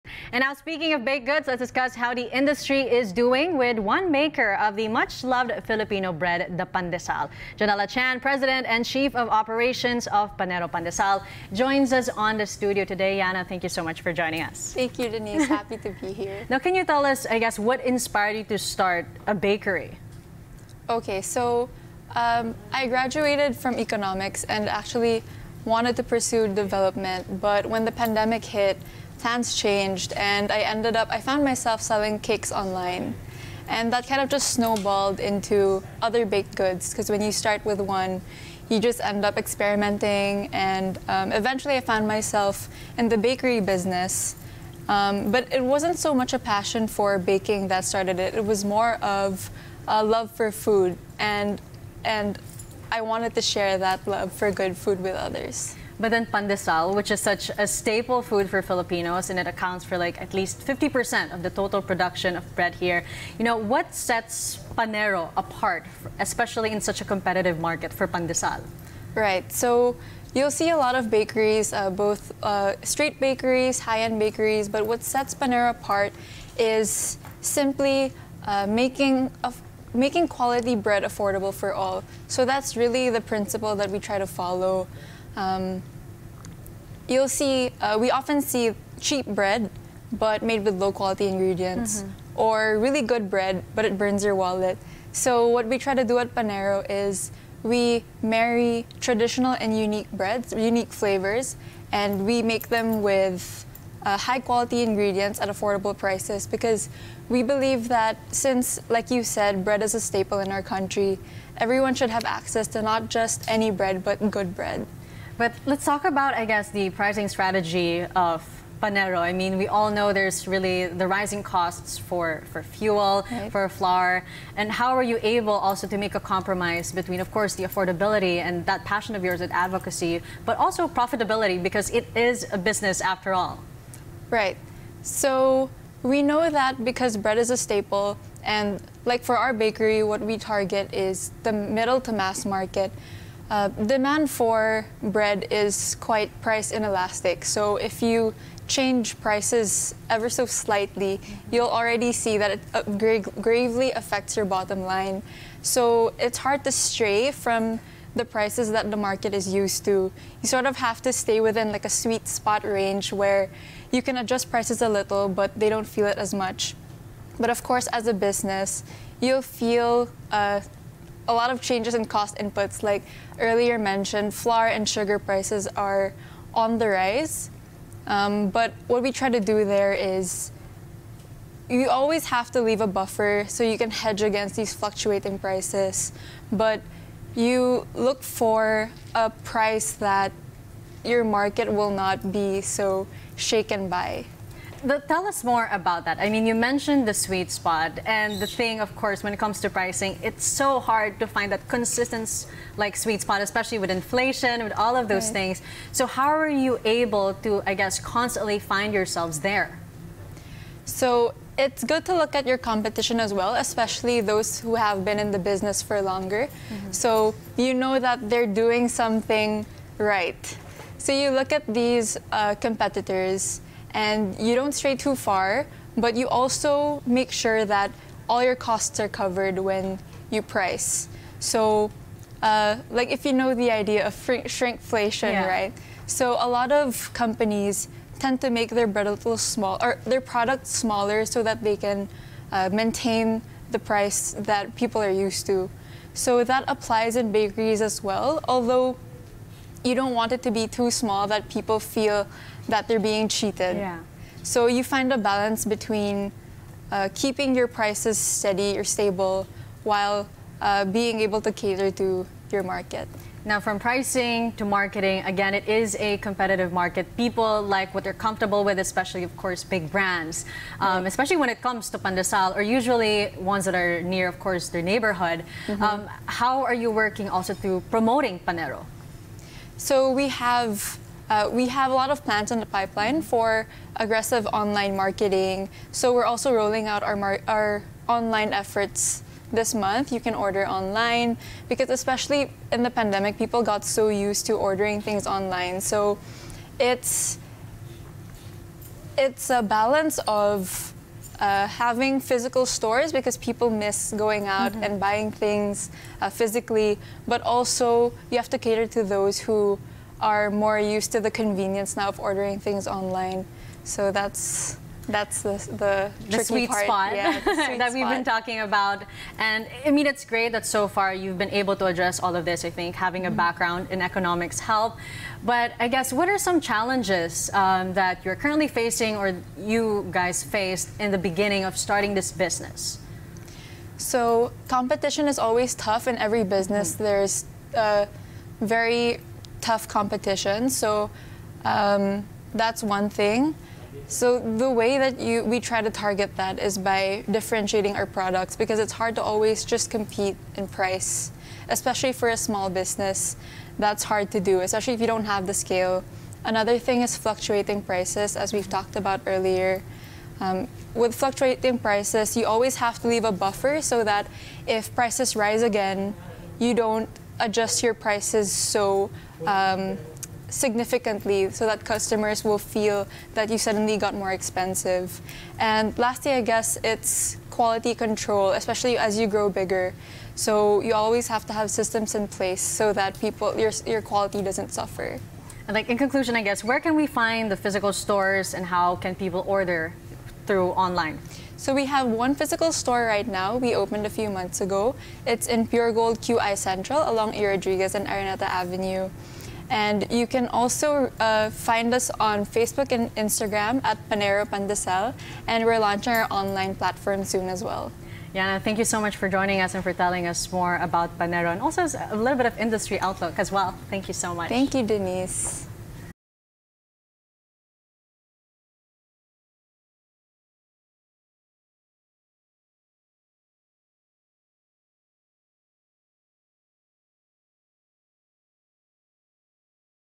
WATCH: Panero Pandesal on having a bakery business | ANC
Business Roadshow: Denice Dinsay talks to Janella Chan, the President and COO of Panero Pandesal.